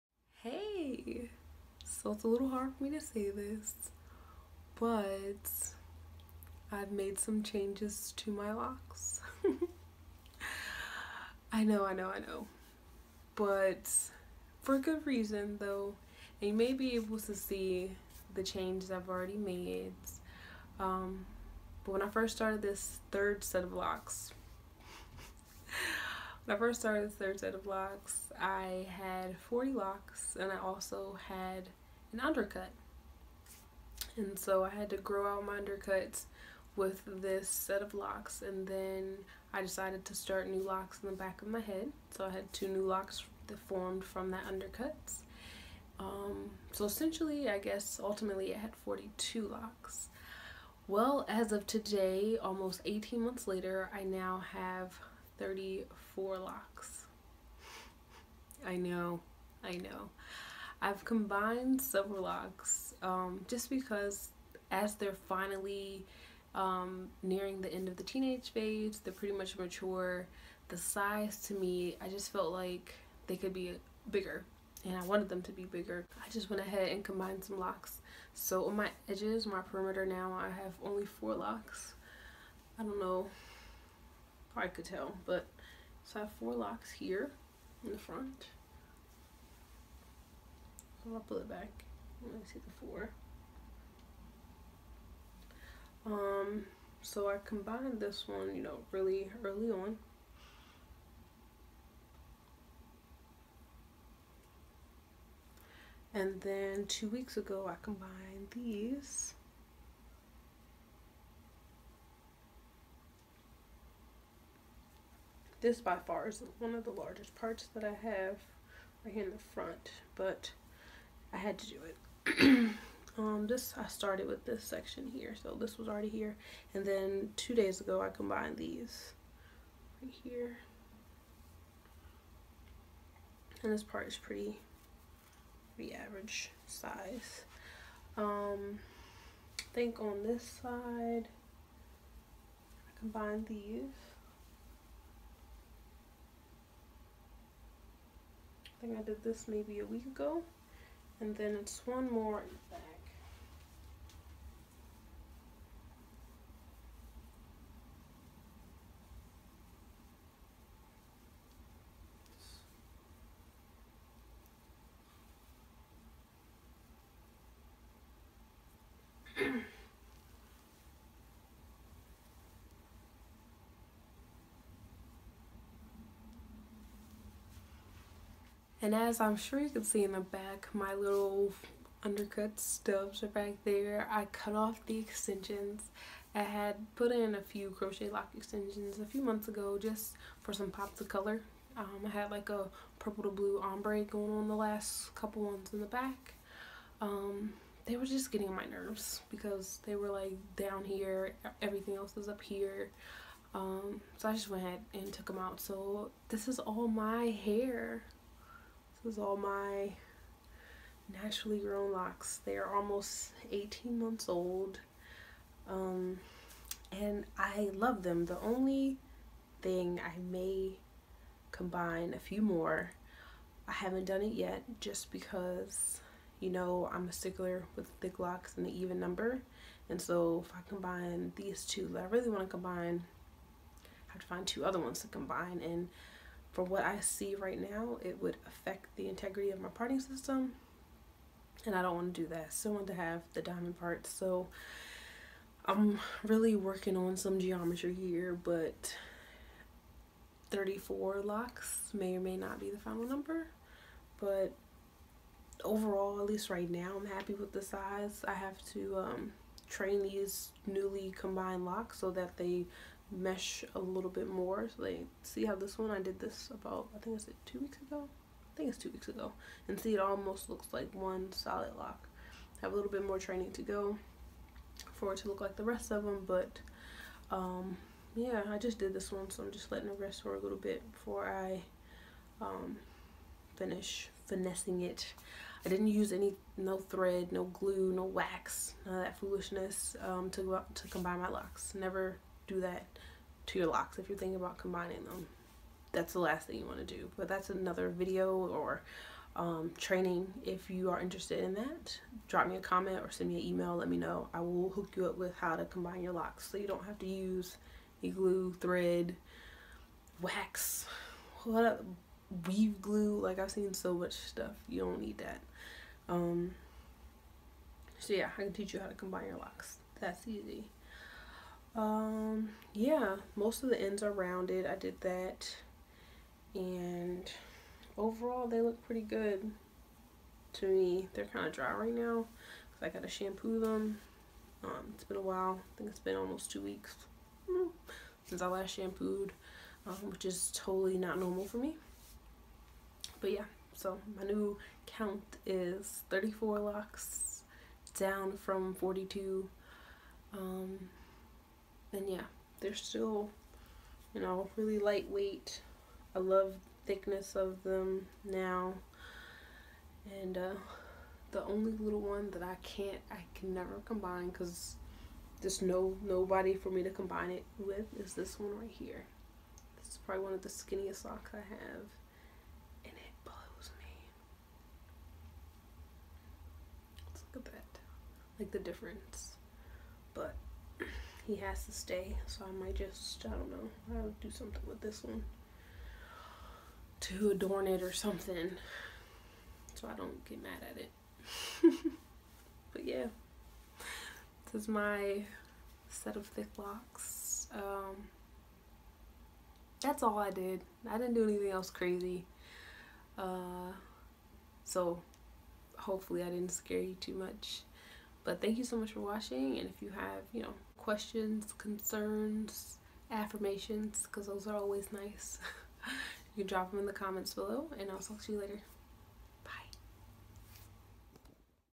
MAJOR changes | 18 Month Thick Locs Update
Major changes happening in this set of locs! I promise I'm not crazy! 😁